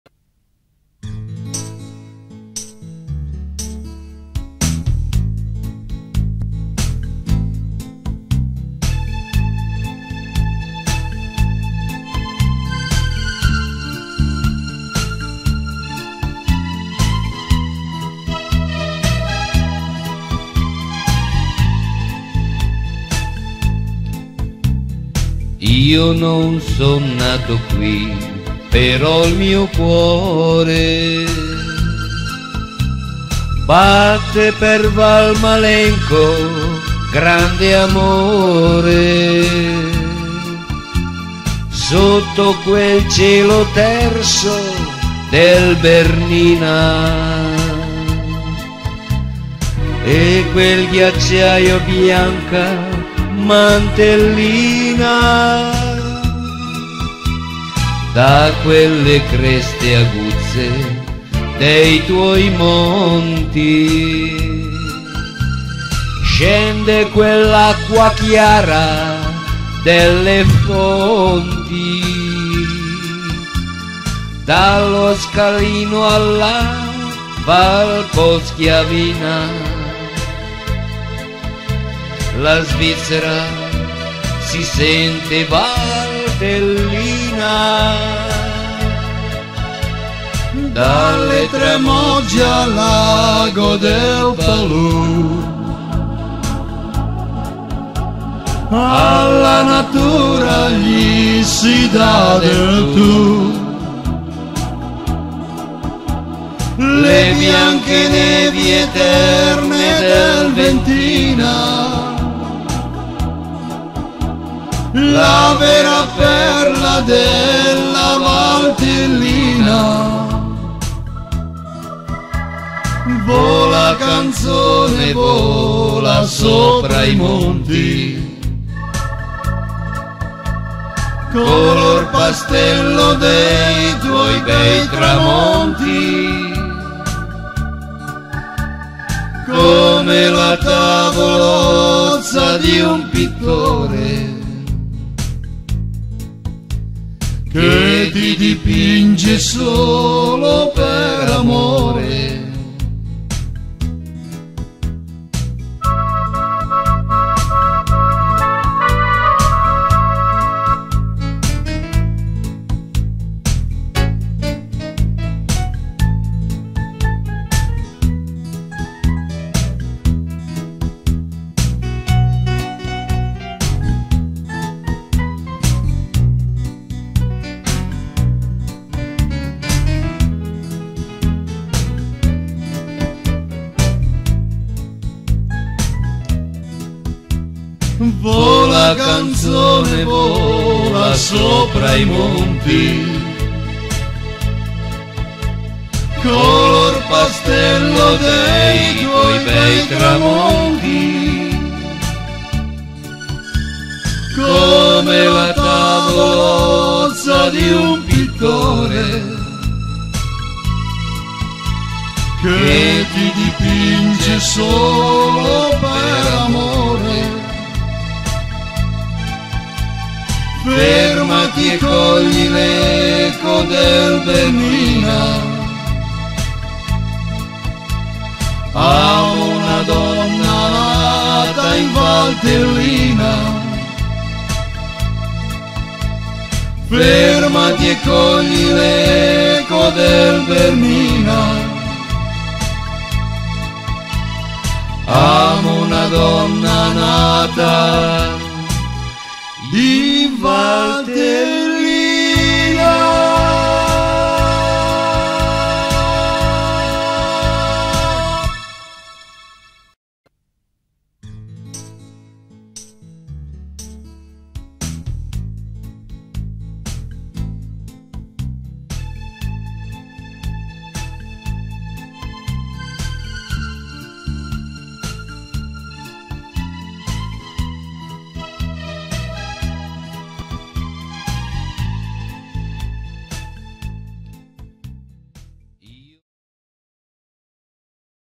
CHIAREGGIO
ALCUNI SCATTI NEI  DINTONI .DI CHIAREGGIO...MUSICA E TESTO  DI GUSTAVO PETRELLI   VOCE   DI GUSTAVO PETRELLI  E CHECCO  PETRELLI   .FOTO E MONTAGGIO..DI...RUFFONI
GIANCARLO. Cbuona visione